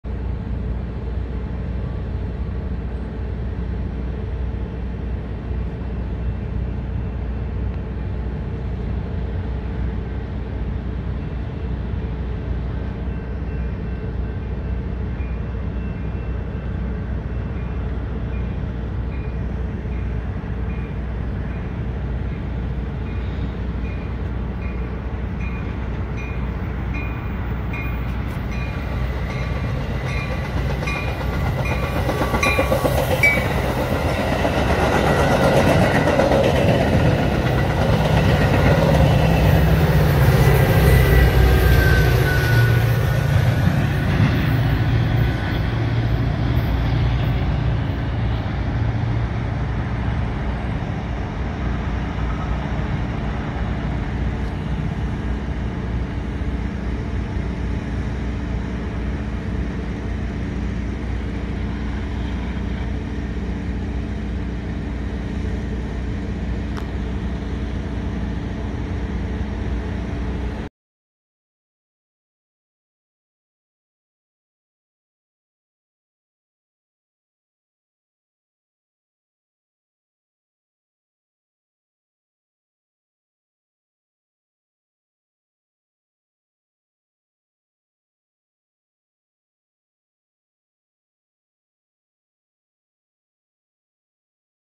Metx 8601 Leads Express Past LaVergne Then Metra 186 & 213 Pushing Go Under CN Freeport Subdivision
Metx #8601 Nippon-Sharyo Coach/Cab leads Express past LaVergne Station, then Metx #186 & #213 both F40PHM-3'S push under the CN Freeport Subdivision on its way to Chicago's Union Station

01/30/2024
BNSF Racetrack
BNSF Chicago Subdivision
LaVergne Metra Station
Canadian National Freeport Subdivision
Berwyn Illinois

This video was part of a previously published compilation